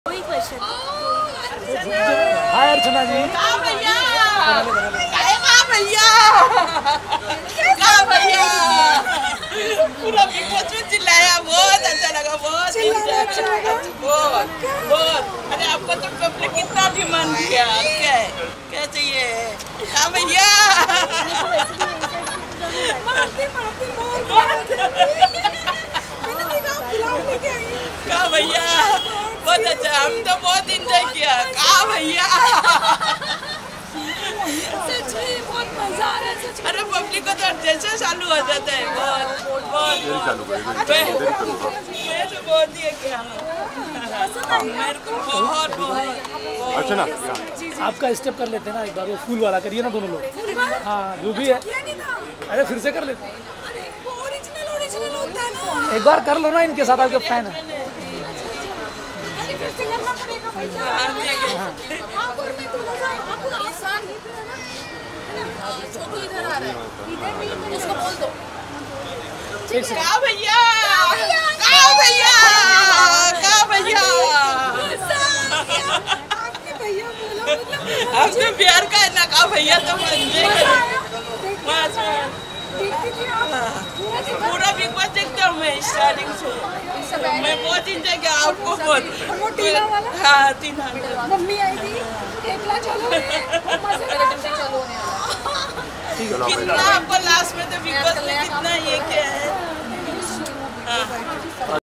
Archana Gautam ने Fan को गले लगाकर किया Kiss !
India Trending News (ITN) : Archana Gautam Hugs and Kisses Her Fan | Archana Gautam Latest News and Updates